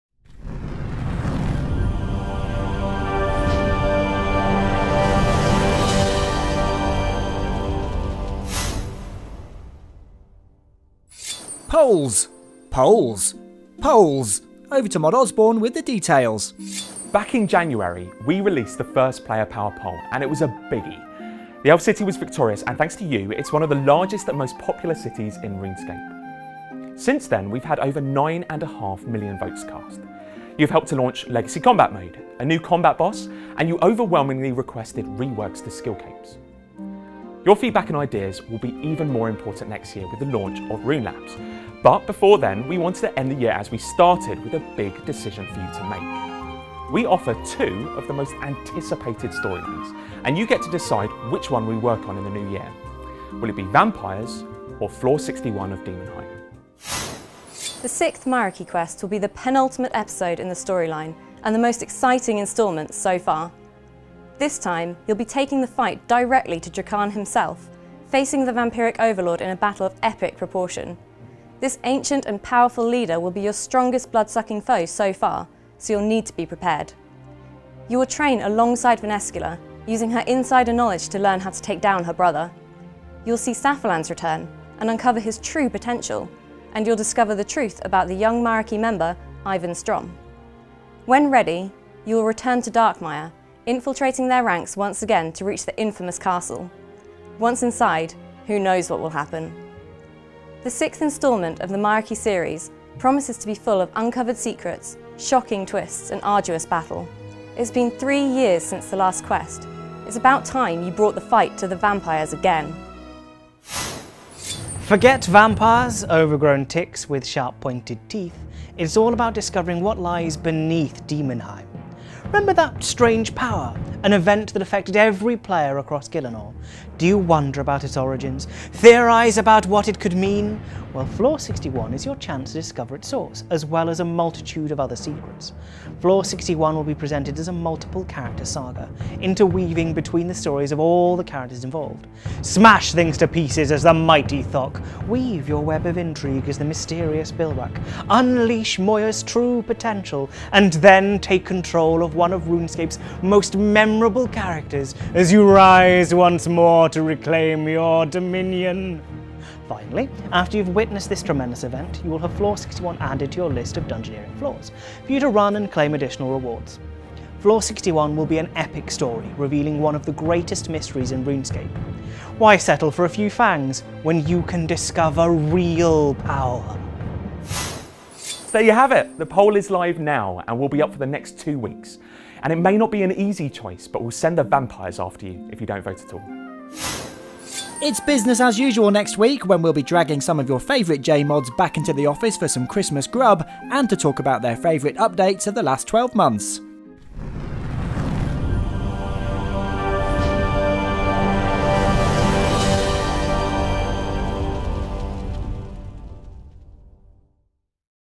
Bastidores em Vídeo 131 - Enquete: Vampiros ou Andar 61
Os Mods Osborne, Ana e Raven precisam de sua ajuda para tomar uma grande decisão nos Bastidores em Vídeo desta semana. Assista agora e fique por dentro!